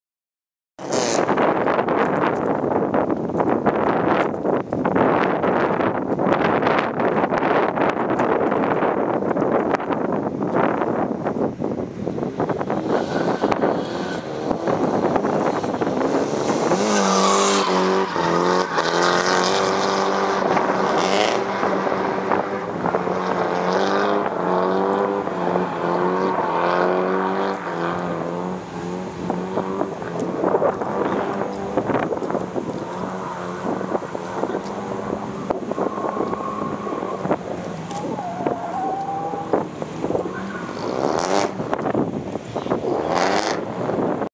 Mark drifting at U Drift - Las Vegas
Mark drifting his car at U Drift in Las Vegas at the LV Motor speedway. He said he wasn't even going that fast. haha fast enough :)